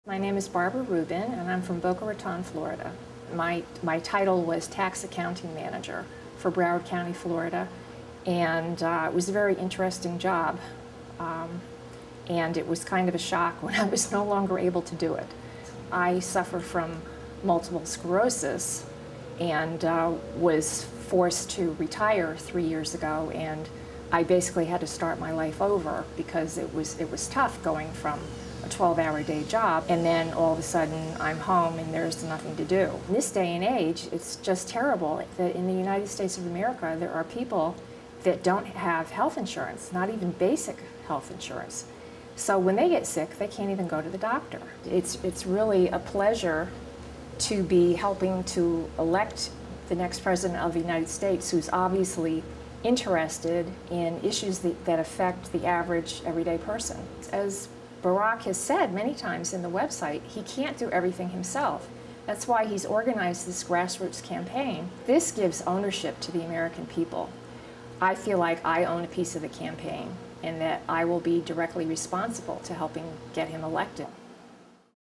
Barack Obama Donor Barbara Rubin
Supporter Barbara Ann Rubin discusses healthcare, and Barack Obama's concern for everyday people.